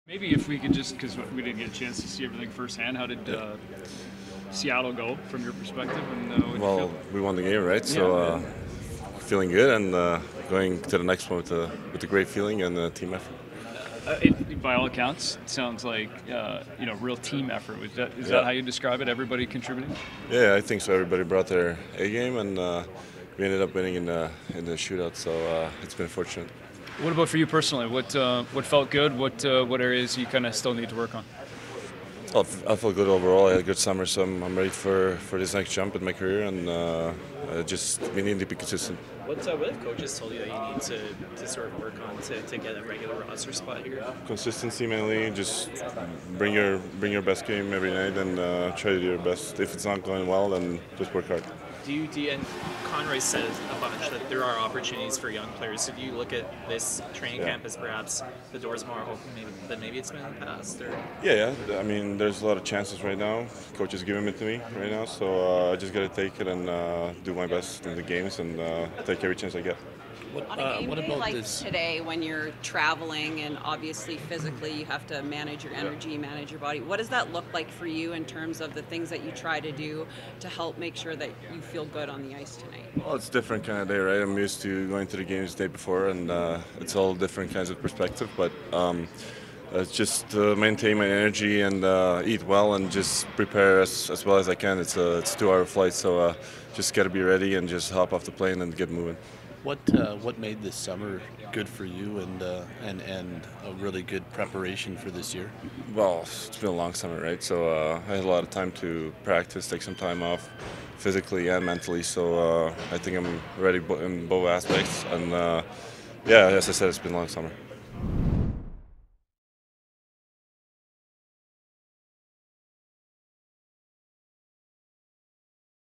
Pre-Game | Ruzicka - 27.09.23
"There's a lot of chances right now, coaches are giving to me ... I've just got to take it, do my best, and take every chance I get."

Adam Ruzicka is out to make an impact in the pre-season.